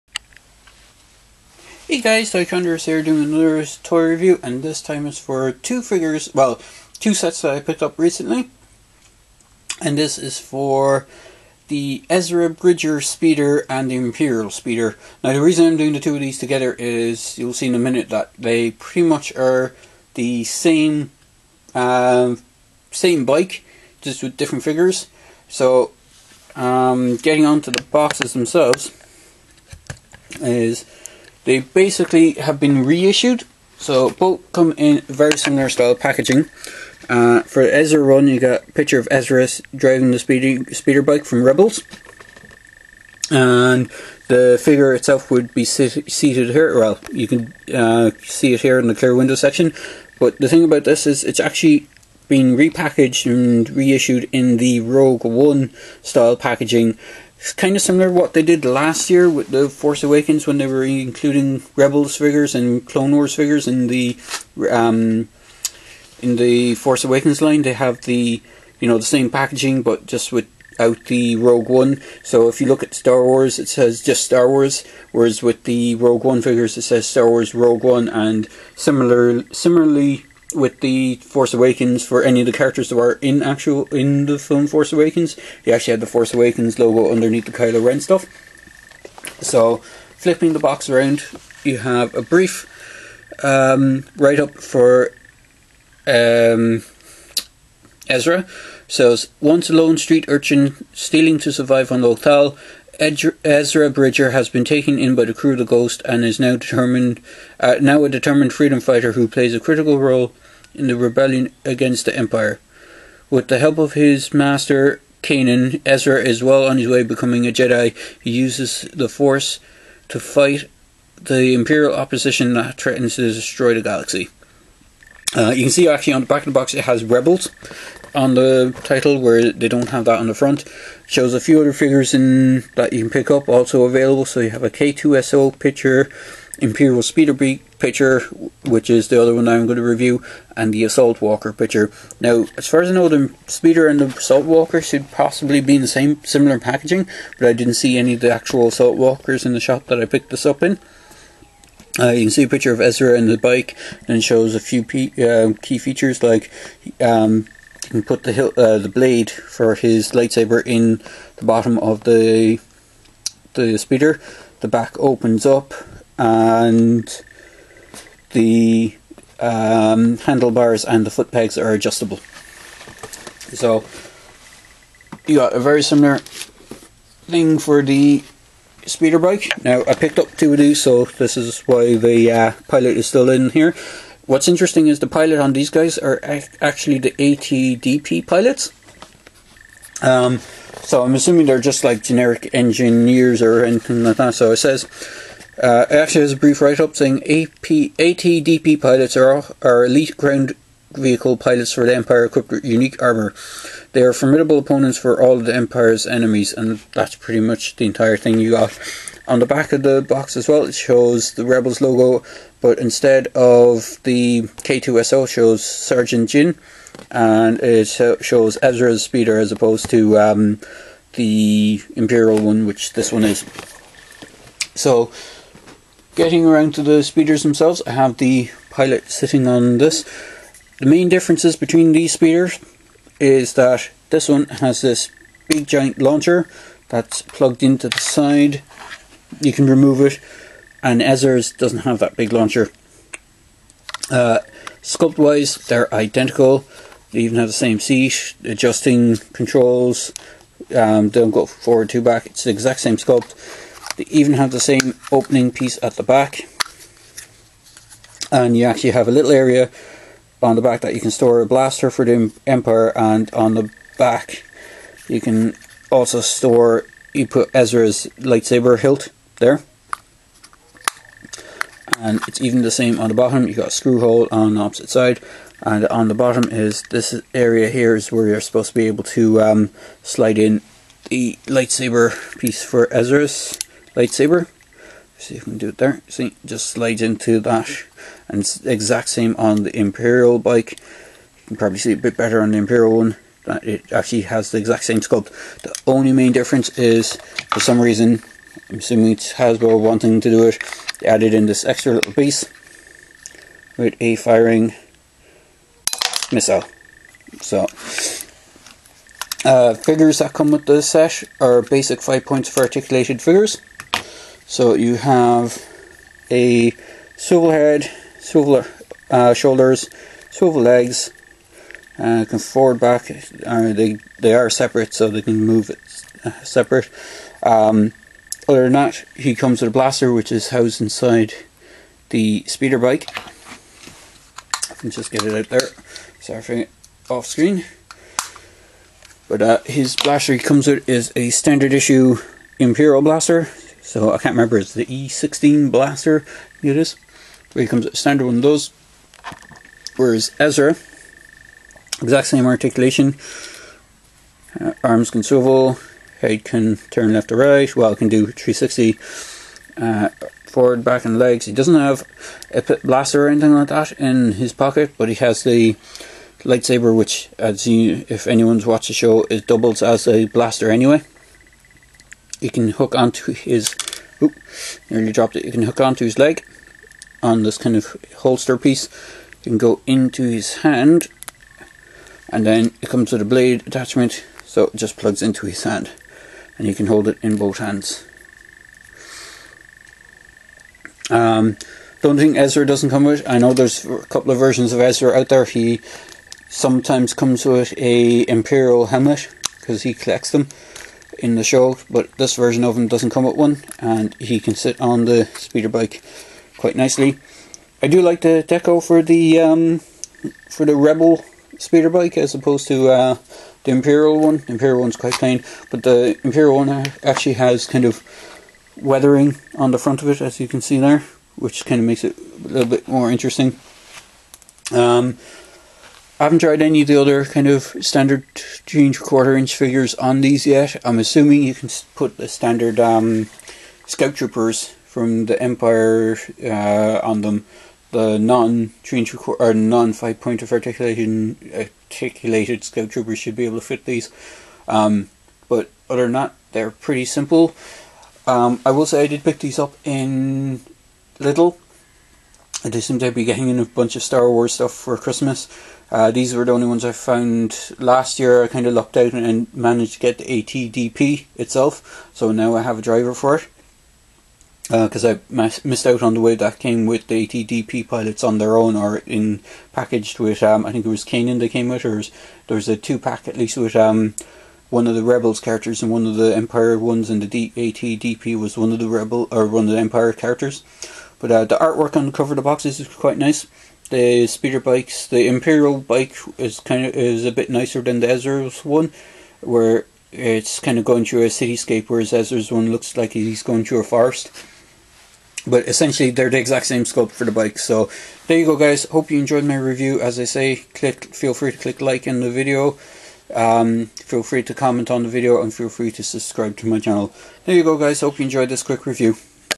Star wars Rebels 2016 speeder bike reissue review
today I take a look see at the two rebels speeders Ezra's speeder bike and the Imperial speeder with AT-DP pilot. these are re-issues from the force awakens line as far as I'm aware.